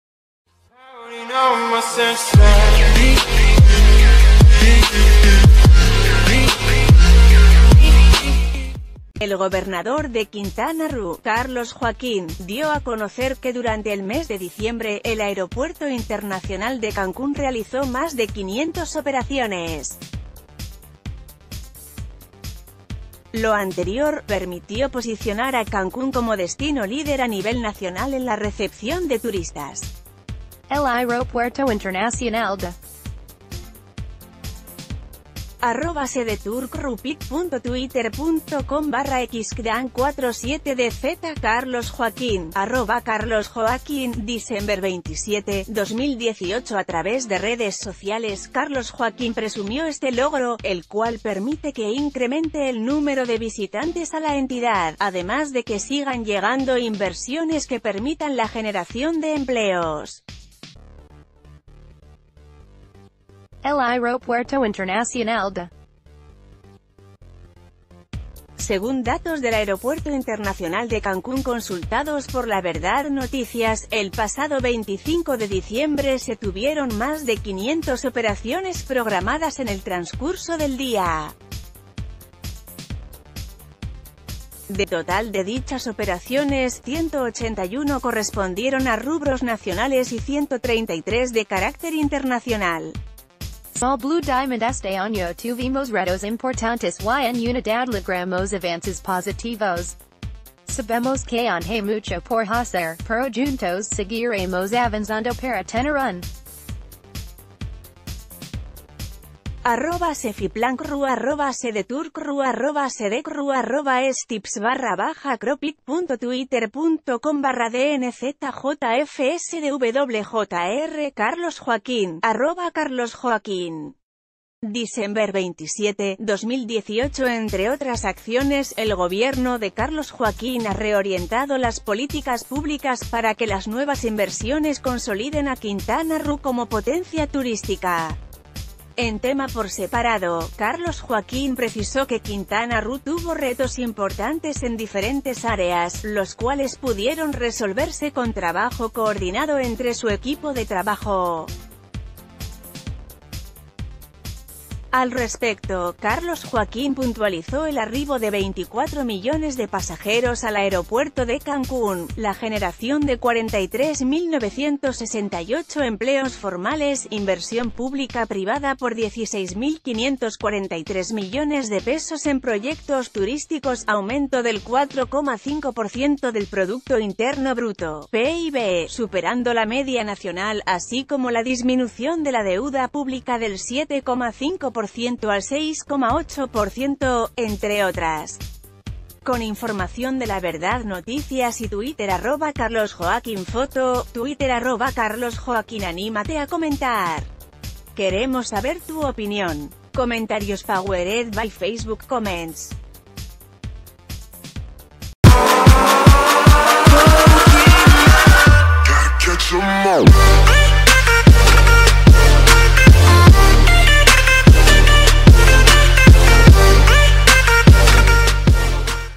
Cancún, líder nacional en recepción de turistas en diciembre: Carlos Joaquín
El gobernador de Quintana Roo, Carlos Joaquín, dio a conocer que durante el mes de diciembre, el aeropuerto internacional de Cancún realizó más de 500 operaciones. Lo anterior, permitió posicionar a Cancún como destino líder a nivel nacional en la recepción de turistas. El aeropuerto internacional d...